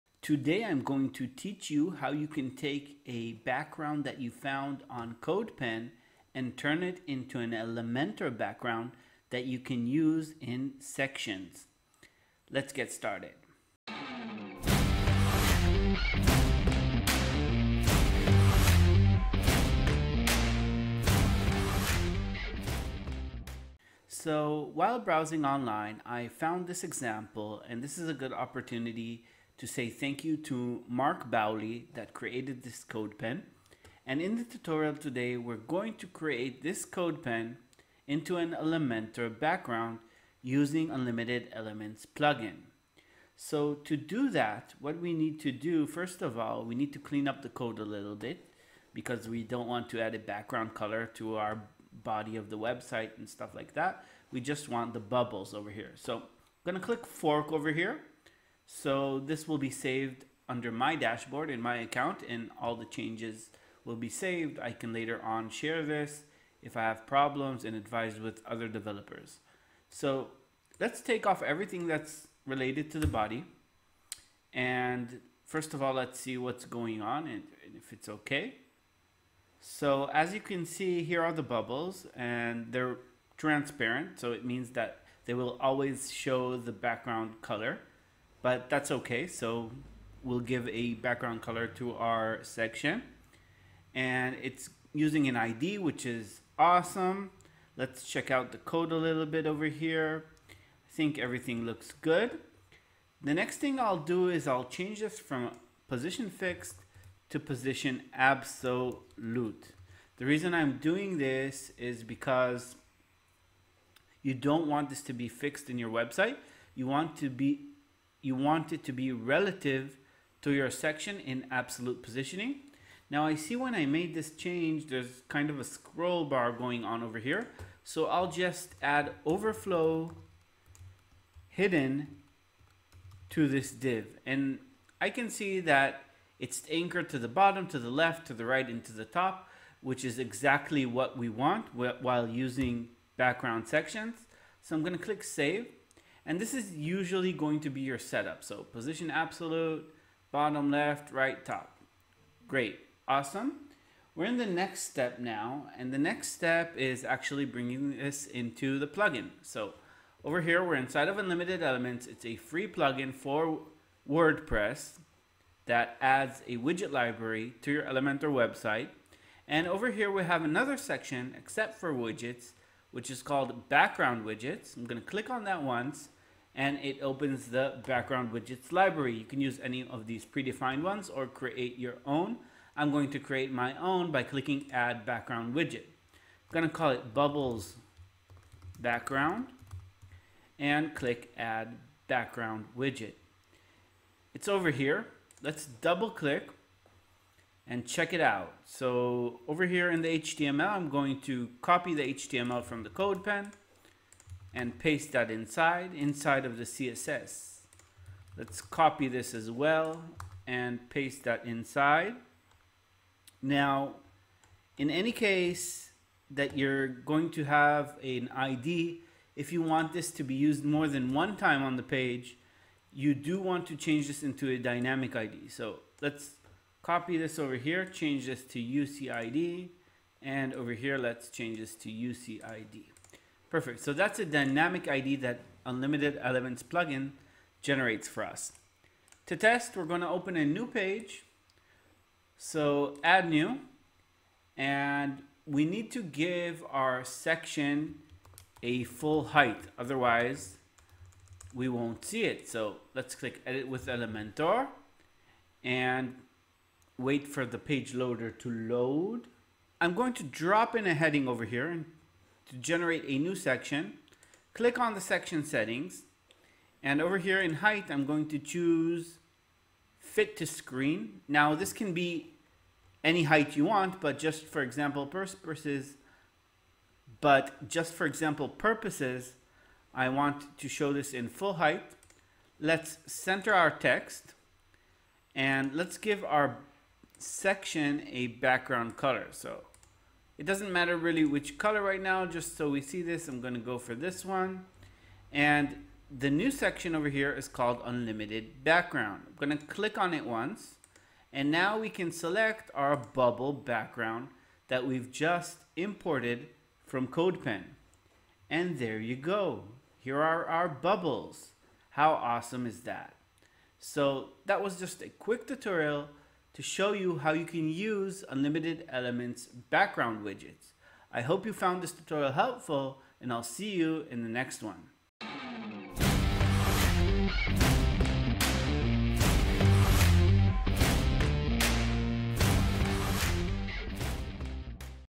Create a Custom Section Background inside of Elementor
In this video i will show you how to create a custom section background inside of Elementor.